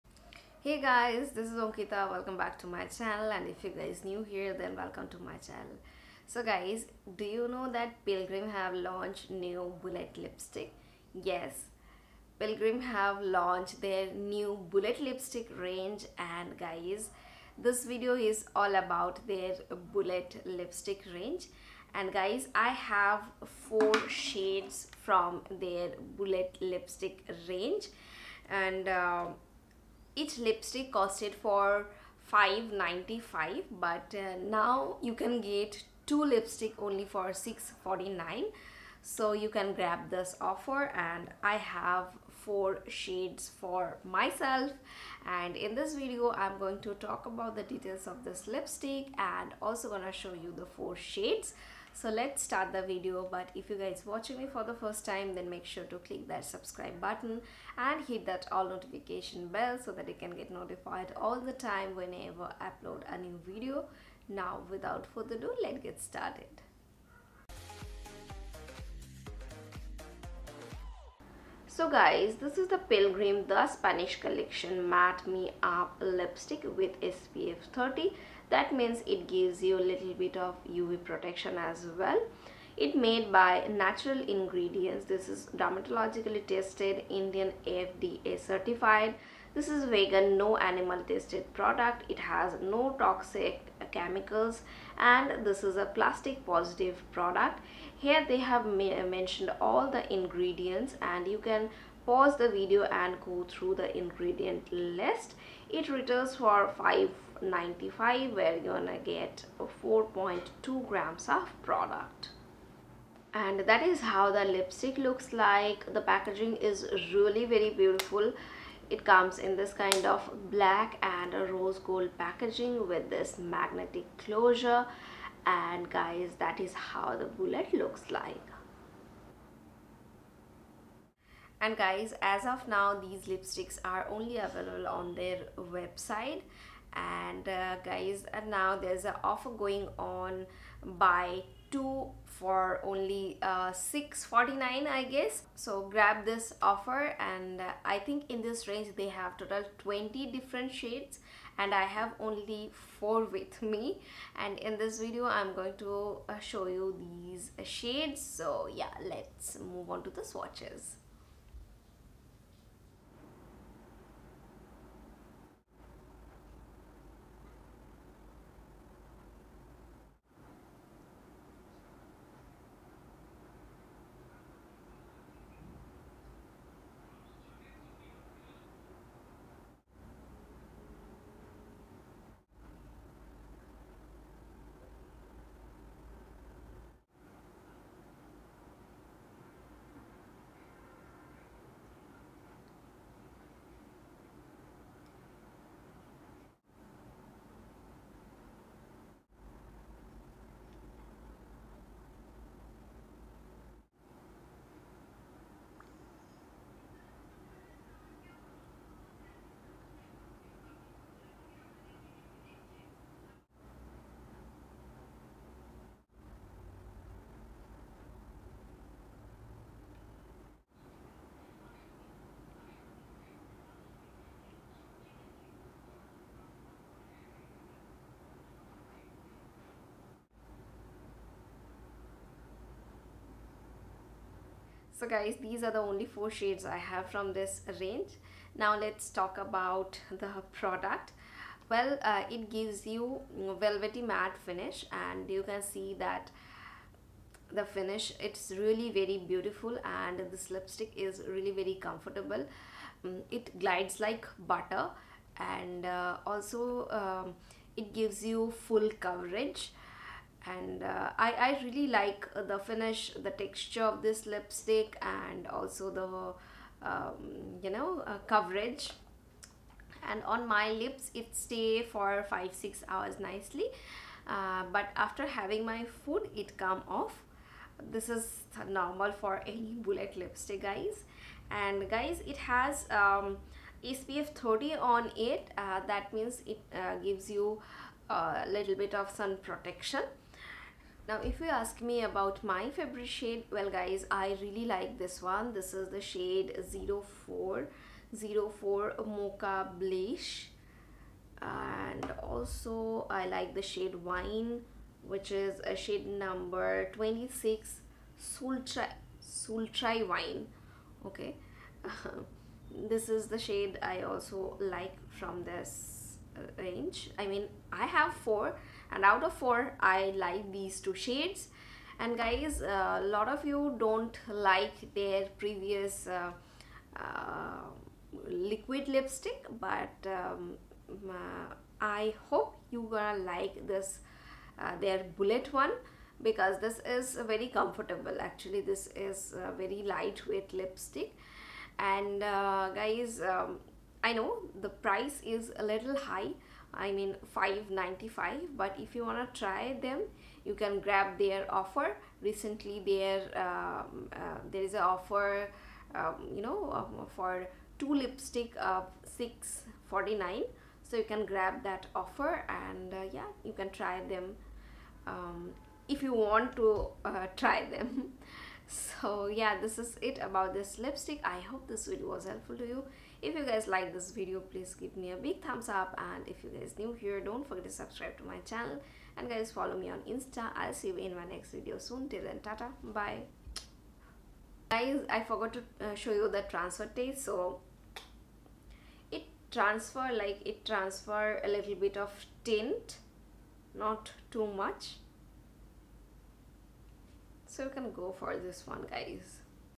Pilgrim Matte Me Up Lipstick with SPF 30| Pilgrim Matte Me Up Bullet Lipstick Review & Swatches
Hi everyone!!!

This video is about Pilgrim matte me  up Lipstick With SPF 30
INR 595/-
Nt Wt: 4.2 g

Pilgrim Matte Me Up Intense Colour Transferproof & Smudgeproof Lipstick
Pilgrim matte me up bullet lipstick
pilgrim matte me up lipstick with SPF 30

My Filming Equipment:
1. Camera:
2. Tripod:
3. Mic: